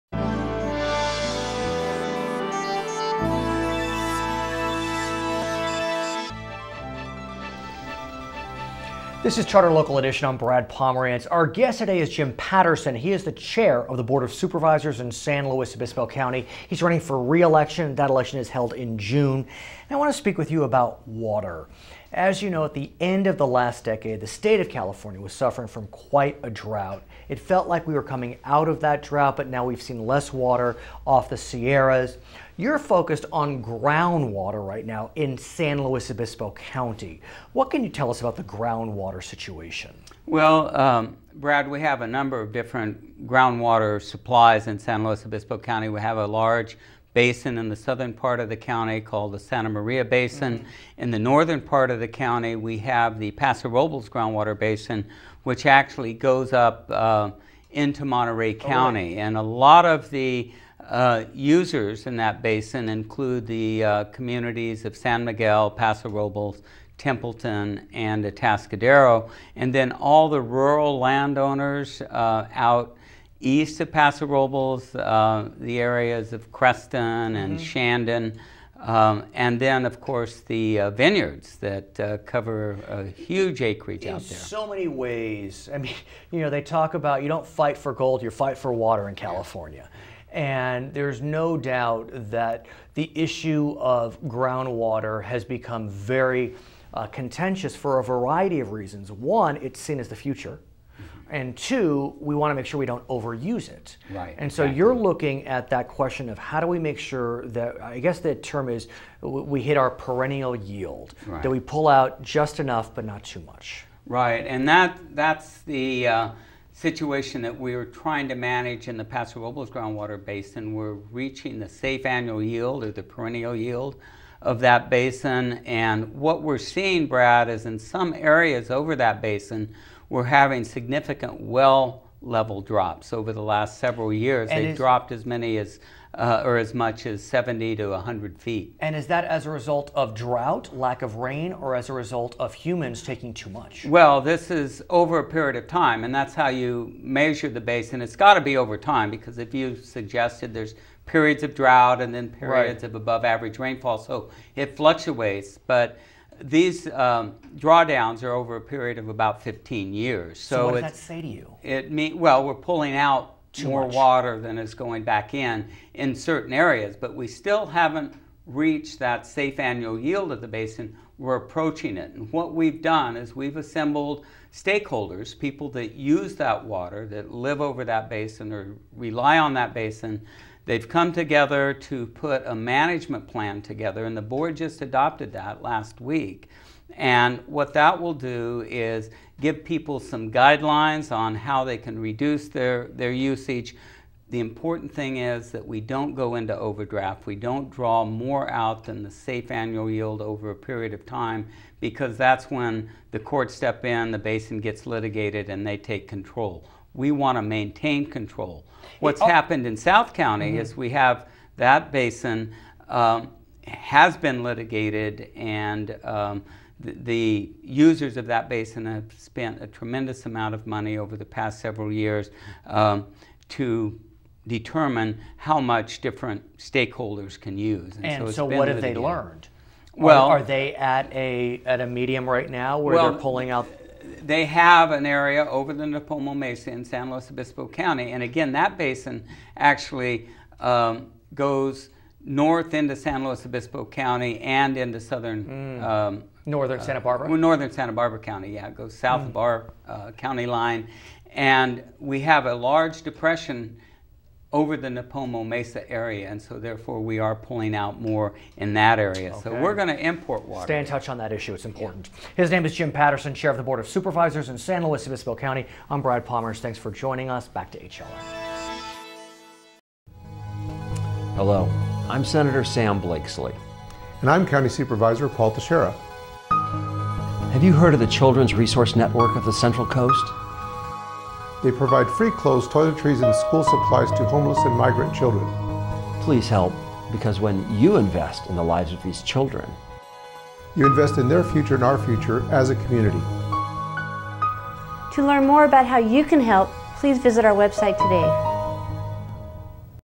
Jim Patterson on Charter Local Edition
Brad Pomerance interviews Assemblymember Jim Patterson about Ground Water